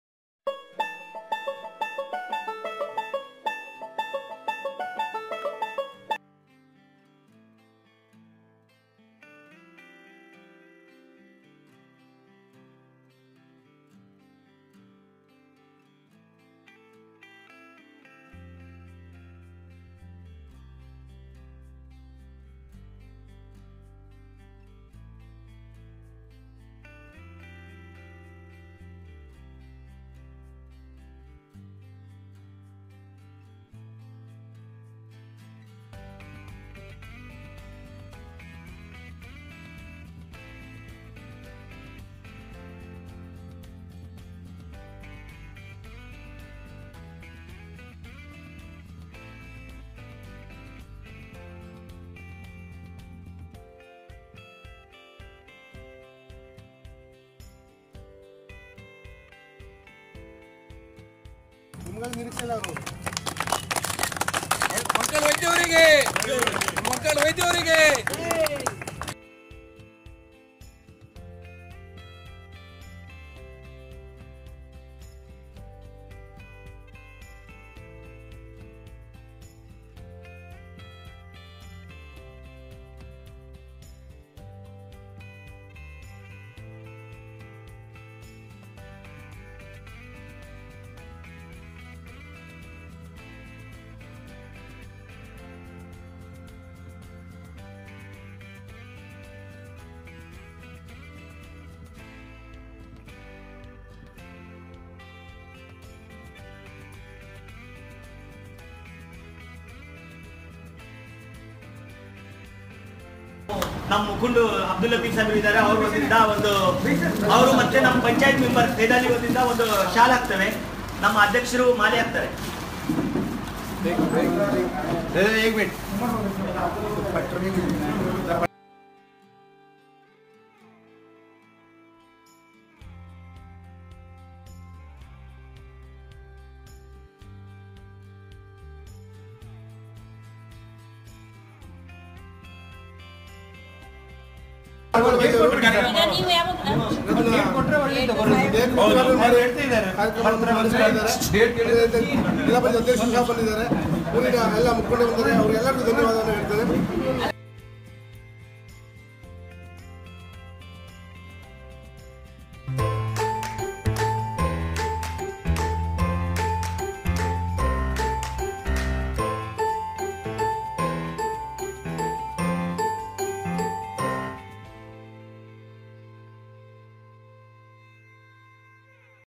MLA Mankal Vaidya Inaugurates Bhatkal Hanifabad Road | Bhatkal Times
MLA Mankal Vaidya
Bhatkal MLA Mankal Vaidya
Bhatkal Honnavar Constituent Assembly